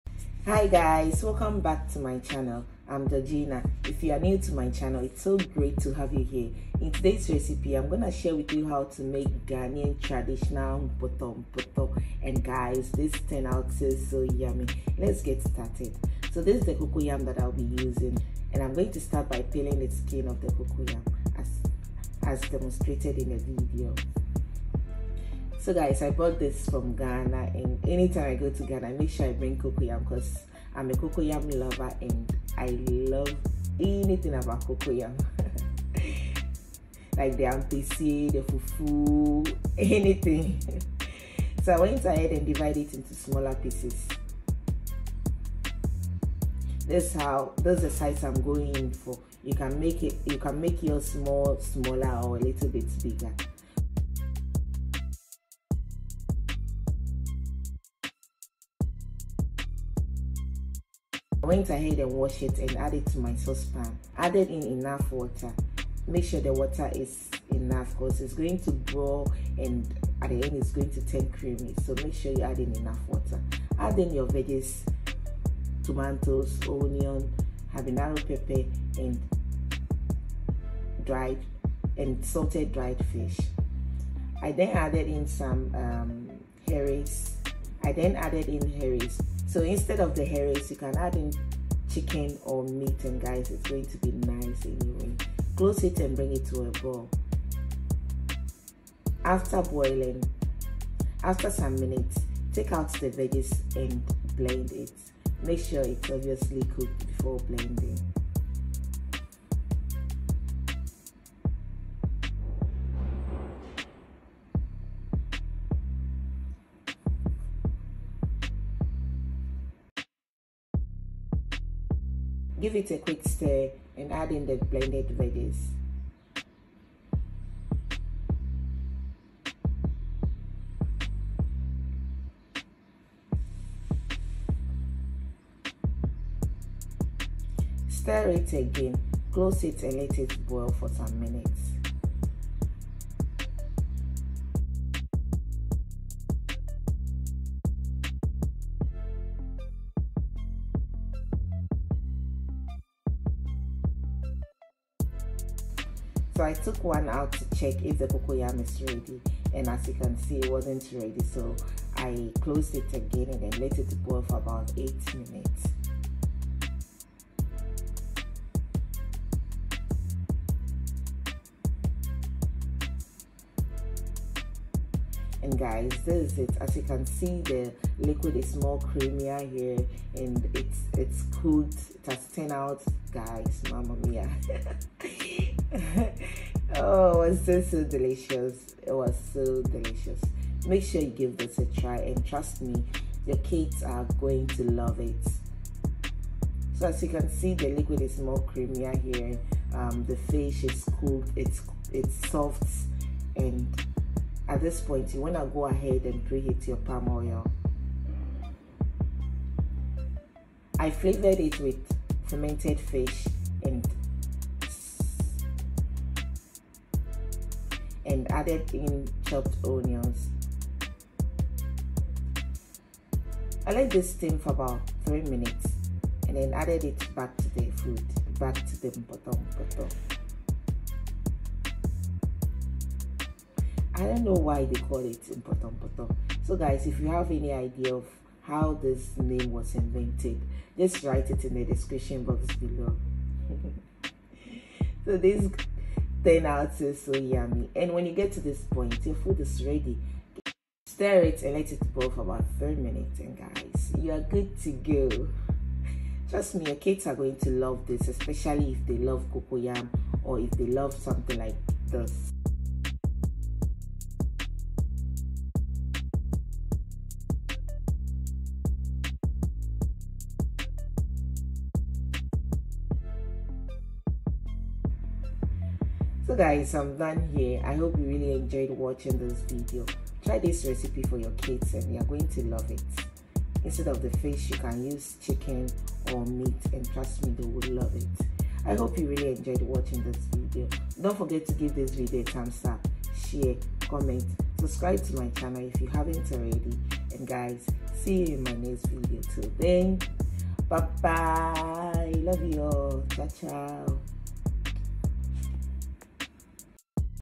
How To Make Cocoyam Mpotompoto
INGREDIENTS
cocoyam
palm oil
smoked dried fish
tomatoes
onion
pepper
fermented fish
and salt to taste